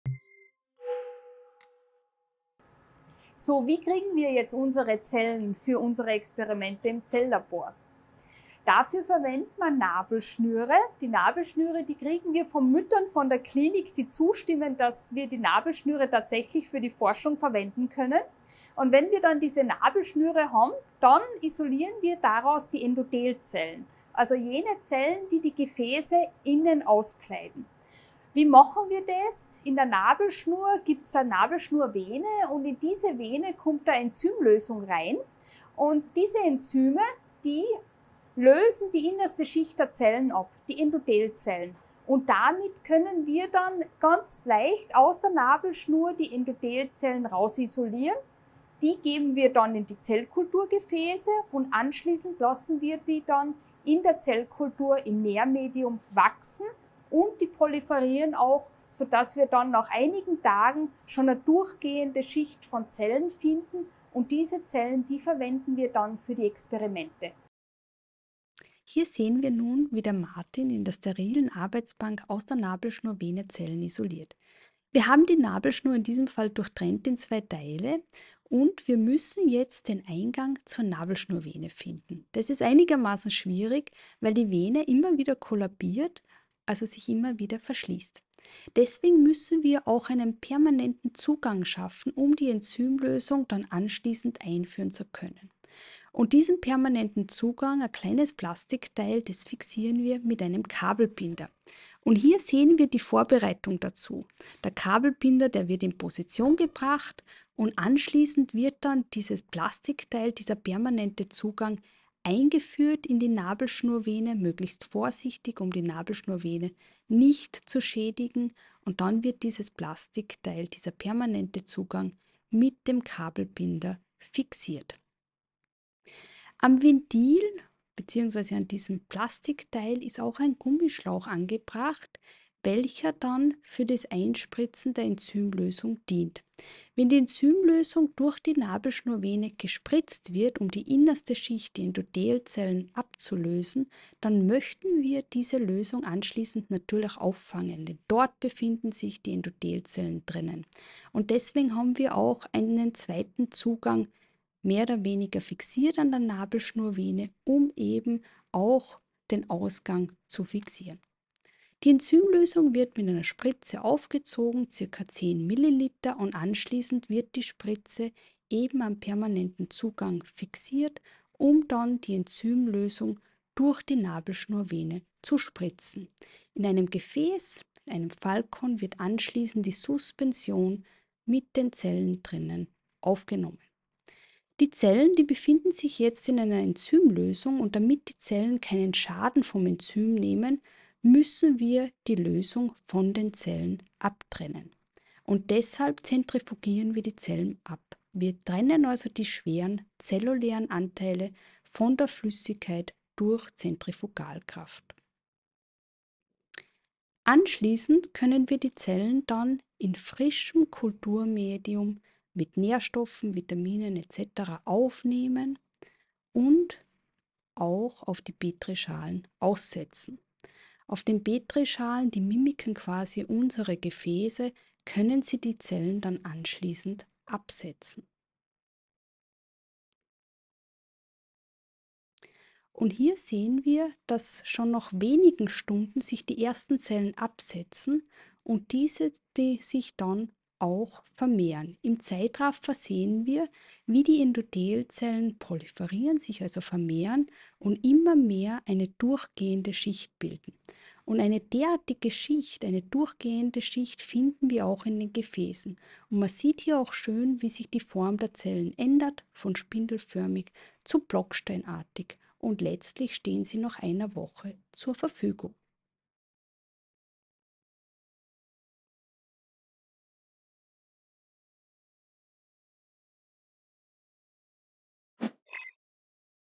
Modul 3 - Zellisolierung | iMooX.at
Im MOOC bekommen Teilnehmer*innen Einblick in gängige Methoden der Alterungsforschung – u.a. in die Kultivierung und Isolierung von Zellen, die Fluoreszenzmikroskopie und die Wirkstofftestung am Fadenwurm.